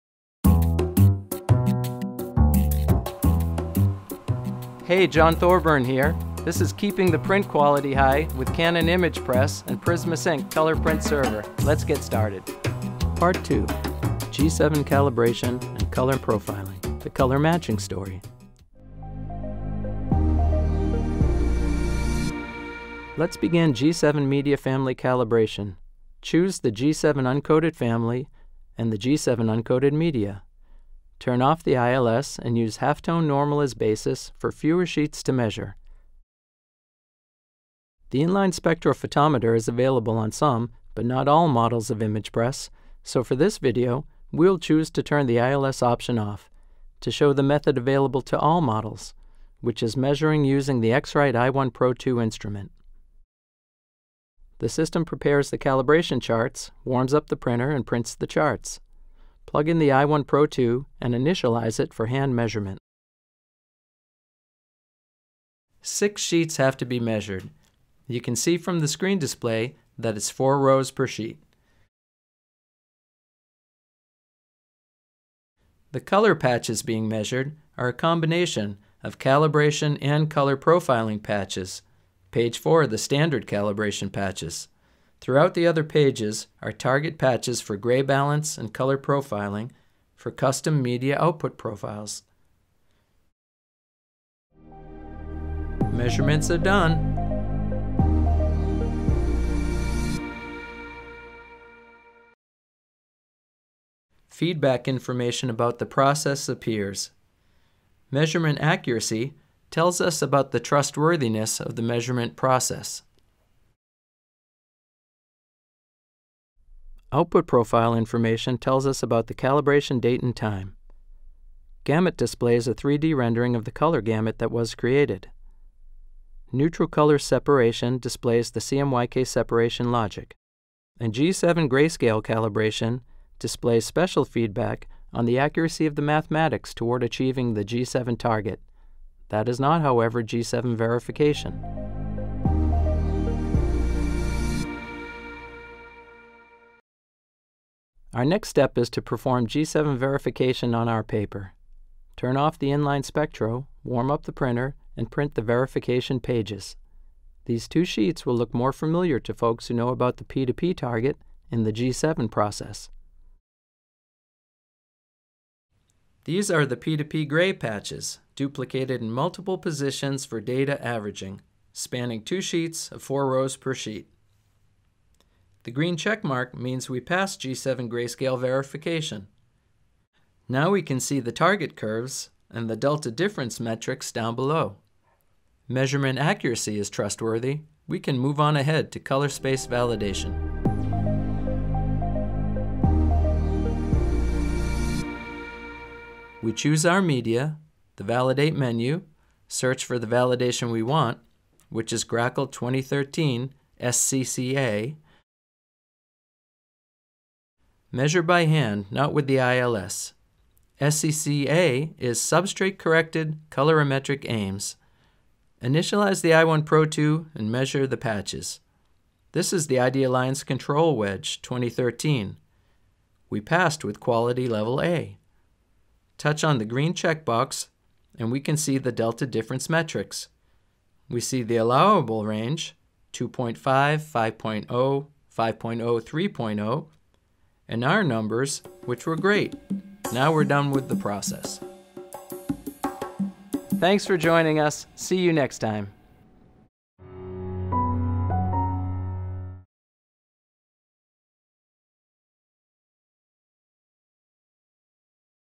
Canon's How to Series: PRISMAsync G7® Calibration and Custom Color Profiles
This video demonstrates how to calibrate a Canon impagePRESS digital color press with PRISMAsync Print Server